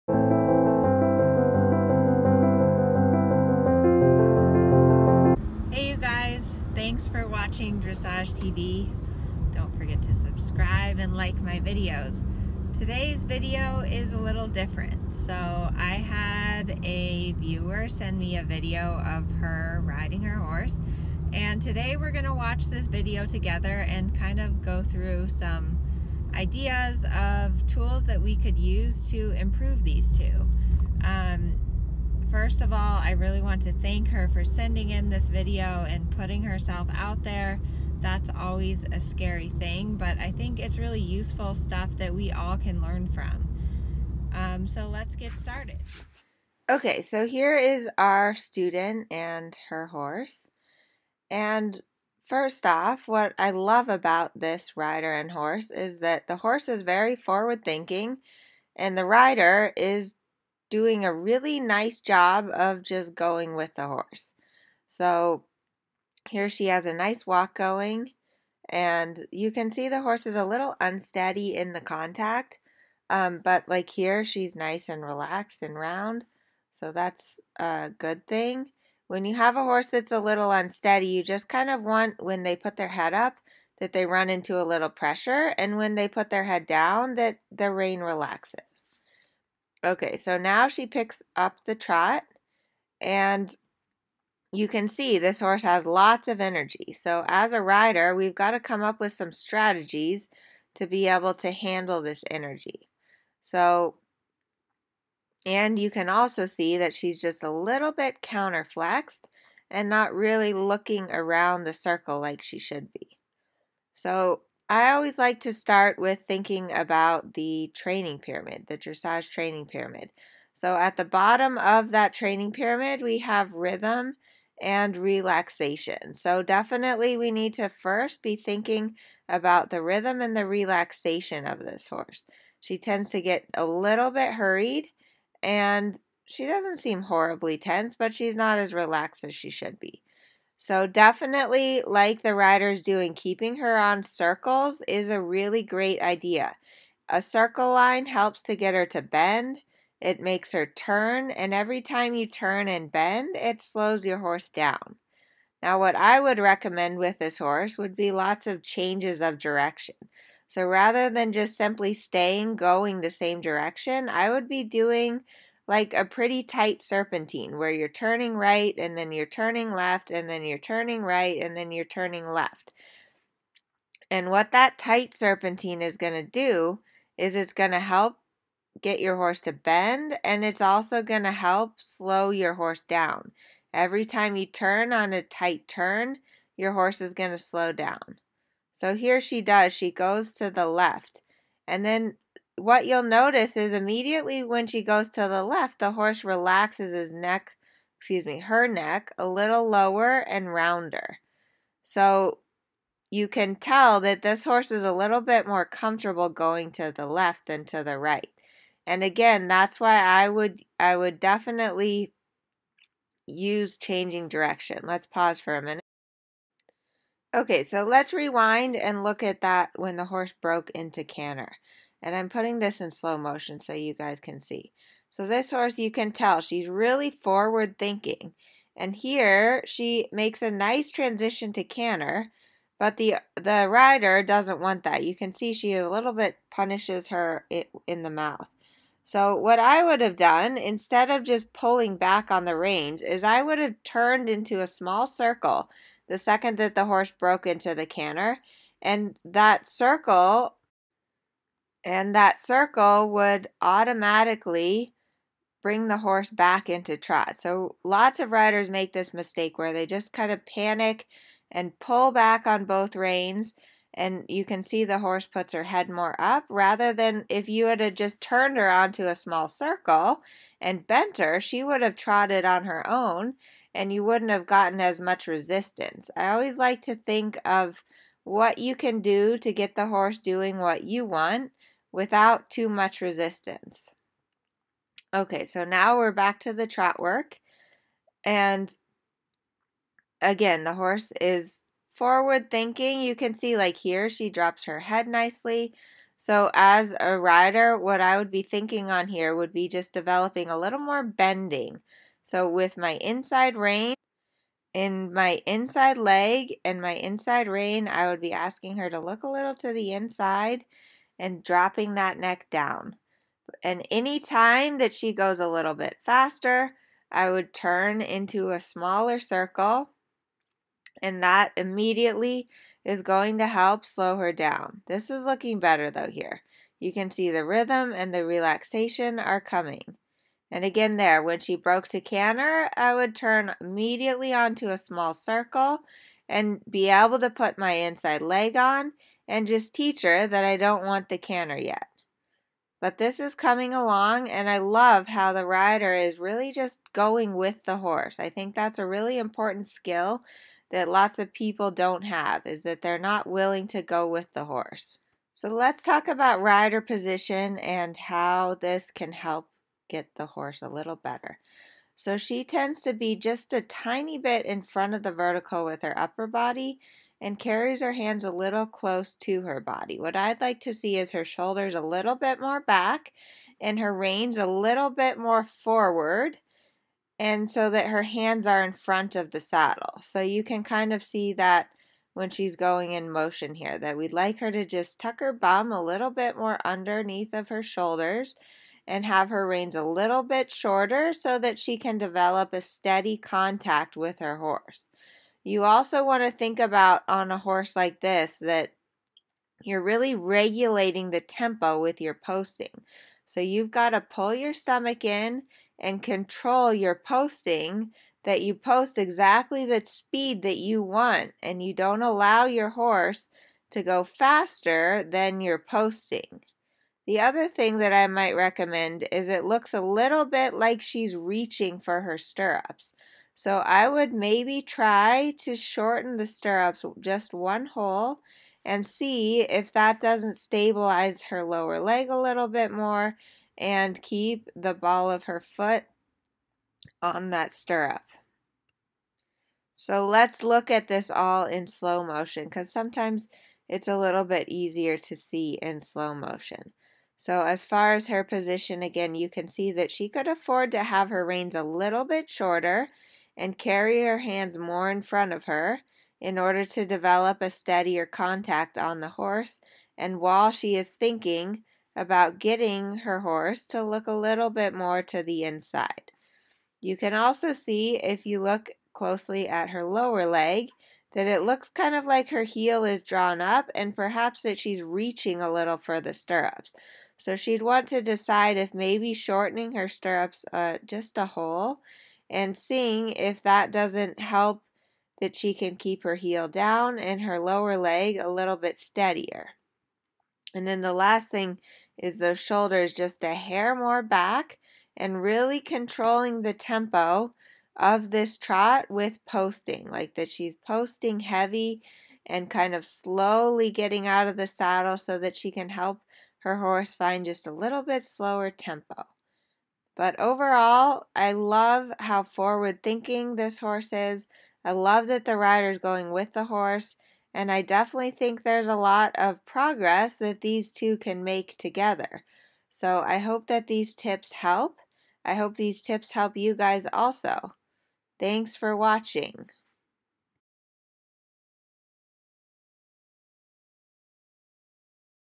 How to slow your horse down and get steadier contact
In this video I have a guest student! We work to regulate tempo on her mare and get more bending. Thank you for submitting this video!